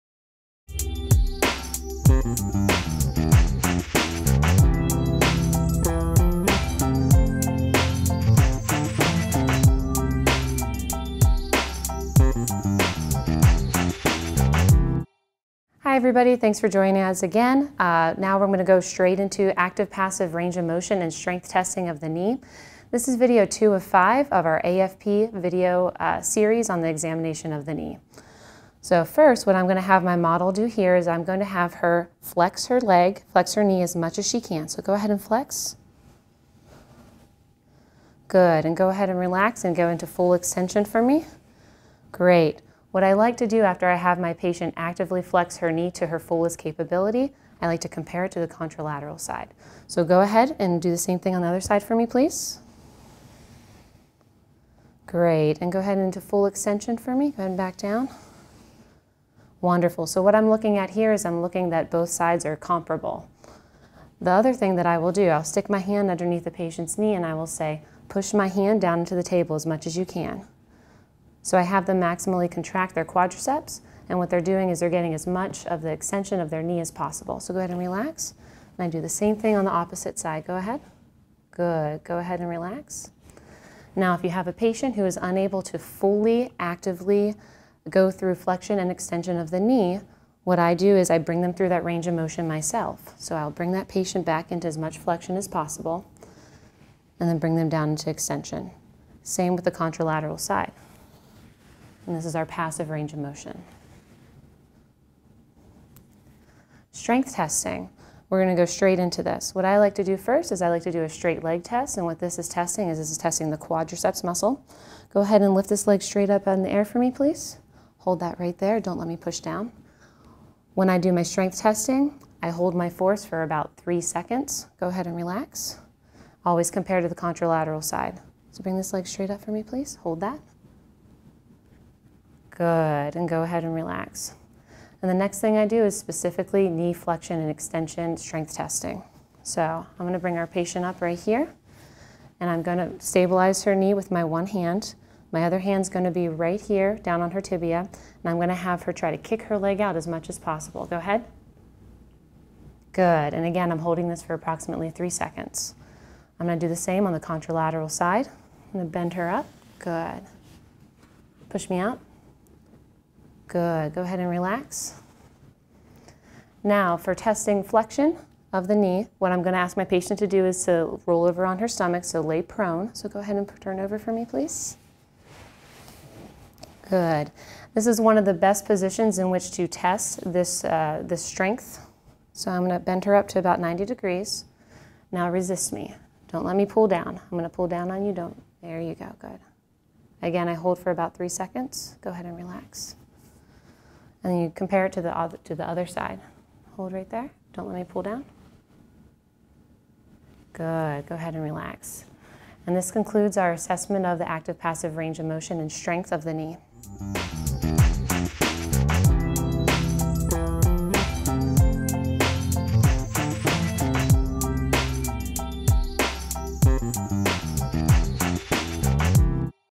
IPASS 2: Knee Examination--Active and Passive ROM and Strength
Video 2 of the IPASS Knee Examination series. Reviews active and passive range-of-motion examination as well as strength testing.